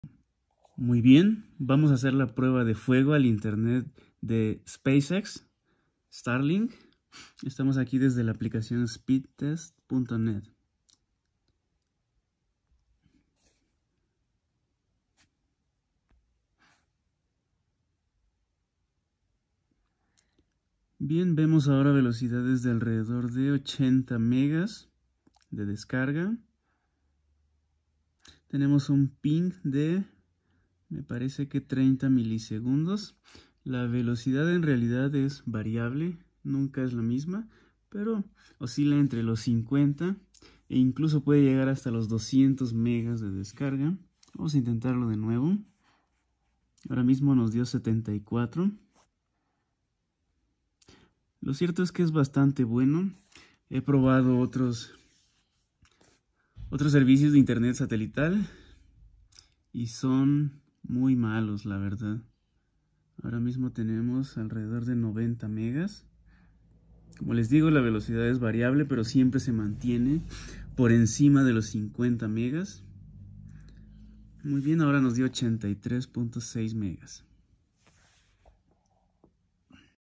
Instalando el internet de STARLINK/ pruebas de velocidad/ Parte 3
Mostrando el funcionamiento del internet de STARLINK desde la aplicación para el celular